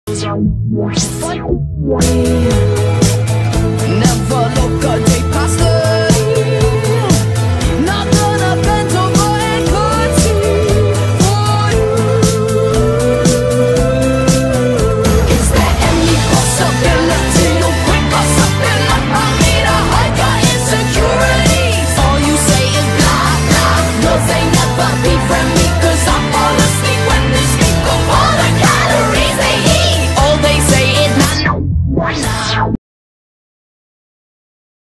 How to fit your heart in a petri dish - Cartoon intro
My first cartoon intro! (Clarifying, I’m not working in the cartoon, it’s just an intro)

Used: Flash CS3, Audacity, Swivel and my mouse because no tablet :c